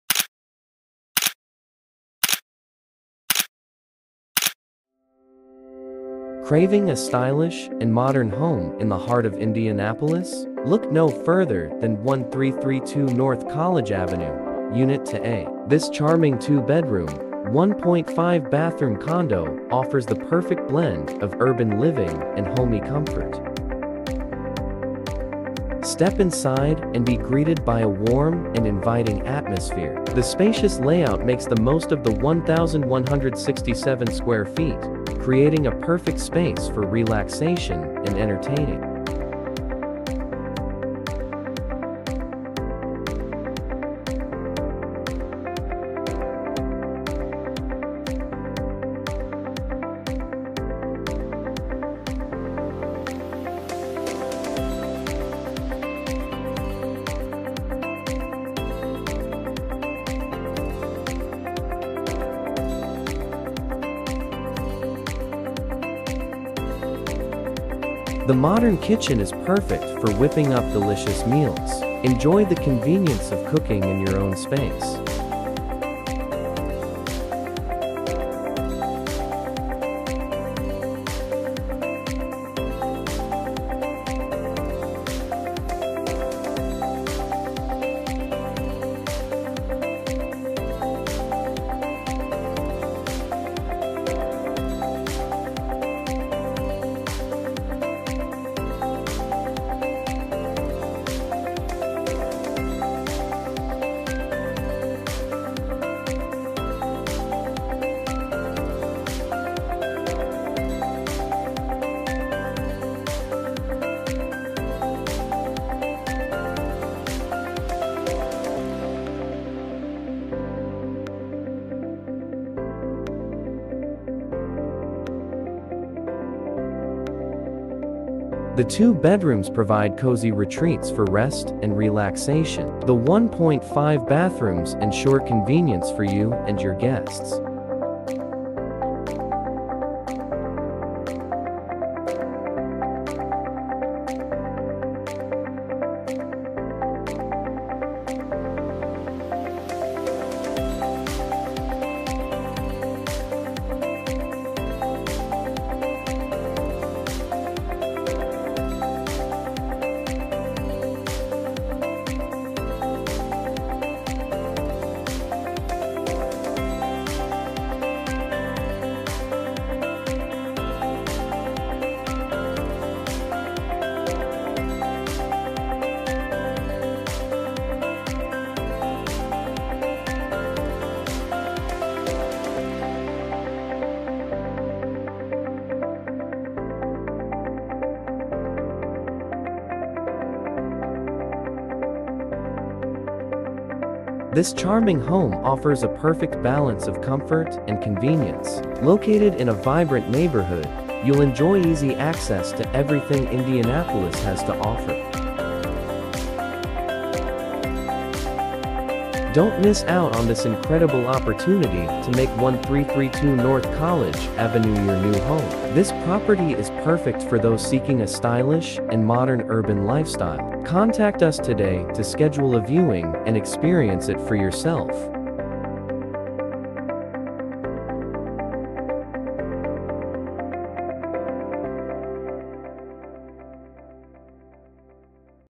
1332 N College Ave - 1332A N College Ave (Virtual Walkthrough) - HOME FOR RENT!
Your Urban Oasis Awaits at 1332 N College Ave!

Craving a stylish and modern home in the heart of Indianapolis? Look no further than 1332 N College Ave, Unit 2A! This charming 2-bedroom, 1.5-bathroom condo offers the perfect blend of urban living and homey comfort.

This charming home offers a perfect balance of comfort and convenience. Located in a vibrant neighborhood, you'll enjoy easy access to everything Indianapolis has to offer.

Don't miss out on this incredible opportunity to make 1332 N College Ave your new home! This property is perfect for those seeking a stylish and modern urban lifestyle.